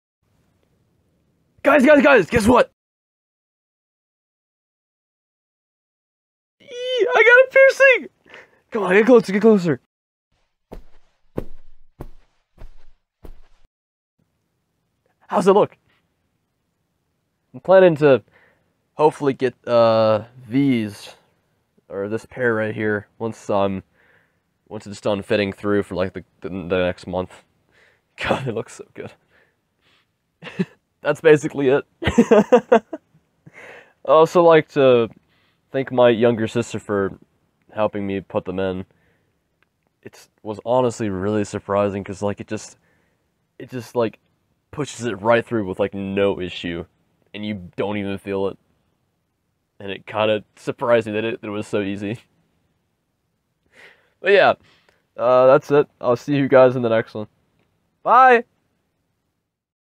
I got a piercing! (IRL)
I finally got my ear pierced yesterday and I had the inspiration to go back and try out the storyboard animation style I had a couple years ago. ^^

Here's some of the studs I'm planning to get when I get my other ear pierced (in case you wanna buy them yourself):